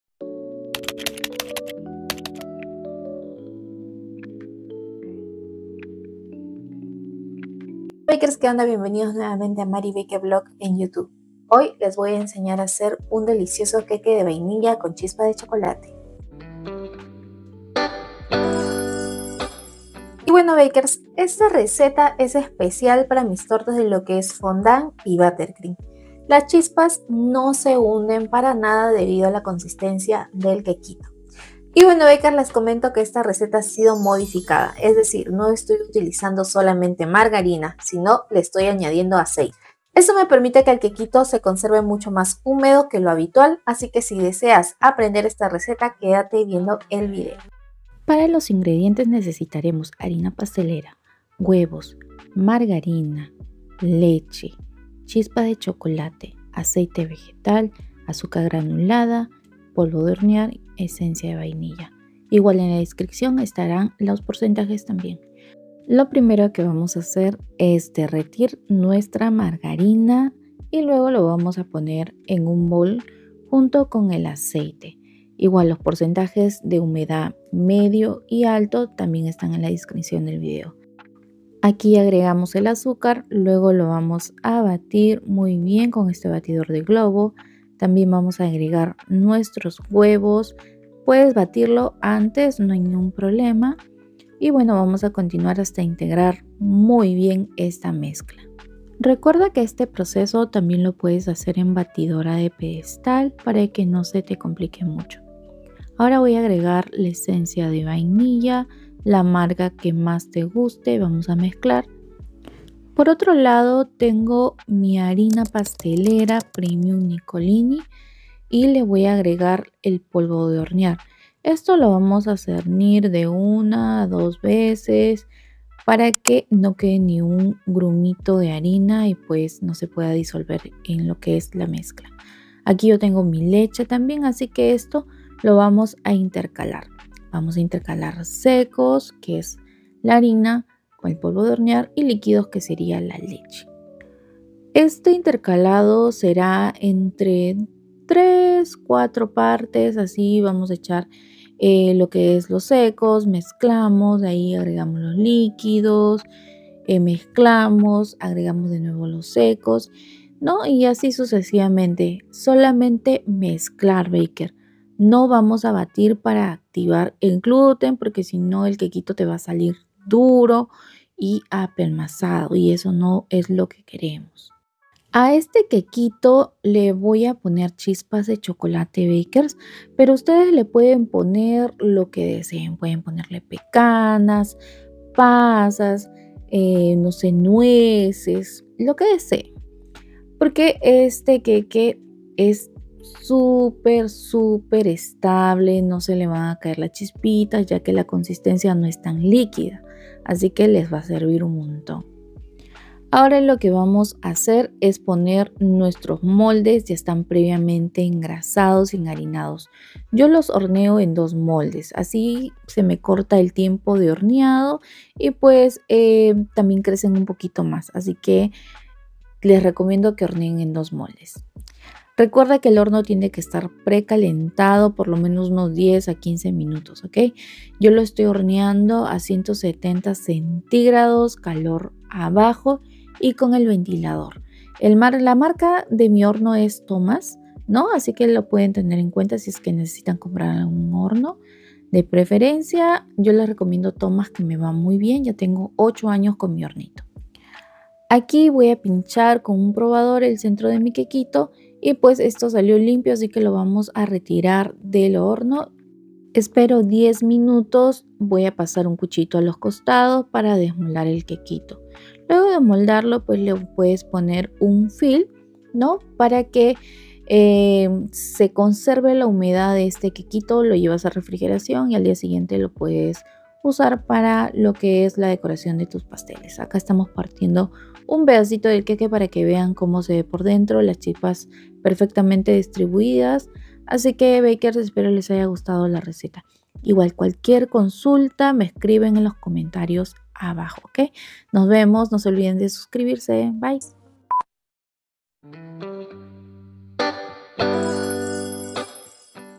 El mejor queque de vainilla con chispas húmedo
Hola Bakers, les comparto esta deliciosa receta de vainilla con chispas de chocolate húmedo con chispas de chocolate... es una modificación a la original que solo llevaba margarina, pero ahora para que sea húmeda hemos reemplazado porcentajes de la materia grasa... les compartiré 3 variaciones donde tendrán un queque húmedo de menor a mayor intensidad dependiendo lo que te guste, teniendo en cuenta que la margarina que uso es sello de oro con un 77% de grasas según la información de su empaque y que al reemplazar en gran parte por aceite, la margarina sello de oro va perdiendo la intensidadde sabor delicioso que le da al queque y se vuelve un sabor más neutro en sabor .

1️⃣ Queque de vainilla  húmedo leve:

 🔸️500g harina pastelera
🔸️25gr polvo de hornear
🔸️193gr margarina
🔸️58gr aceite
🔸️5 huevos
🔸️250ml leche fresca
🔸️250gr azúcar granulada
🔸️1cda esencia de vainilla
🔸️200gr chispas de chocolate

2️⃣ Queque de vainilla húmedo medio:

🔸️500g harina pastelera
🔸️125gr margarina
🔸️97gr aceite
🔸️5 huevos
🔸️250ml leche fresca
🔸️250gr azúcar granulada

3️⃣ Queque de vainilla húmedo intenso:

🔸️500g harina pastelera
🔸️200gr aceite
🔸️5 huevos
🔸️250ml leche fresca
🔸️250gr azúcar granulada

y bueno bakers, como les menciono esta receta alcanza para un pastel de 20cm de diámetro en tamaño regular de 10cm de alto o un pastel doble altura de 20cm de alto en 16cm de diámetro 😊

Pdata: estamos resubiendo algunoa videos porque estamos editando el audio y la edicion de video para que tenga mejor calidad 😊🩷 porque ya aprendi a editar mejor 🤭